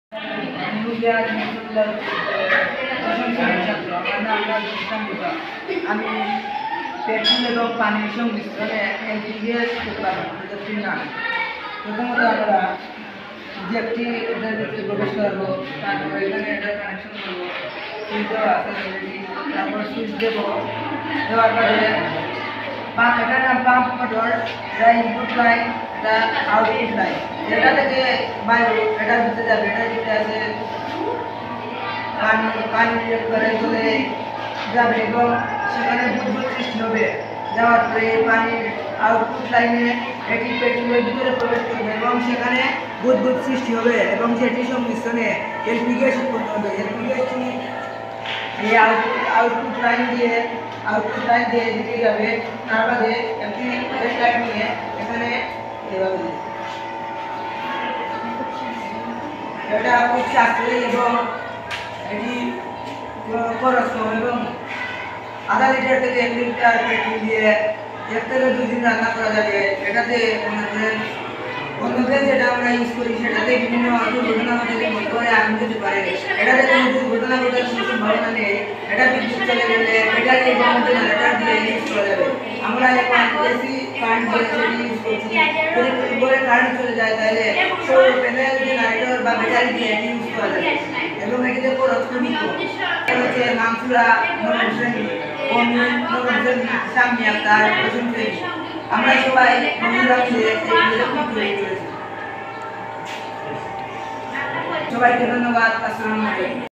Project name : L.P.G Gus
Project Present By: Jhon Dalton Group
School Name: Huglia Hagi Monsob ullah High School.
School Base science fair and quiz 2022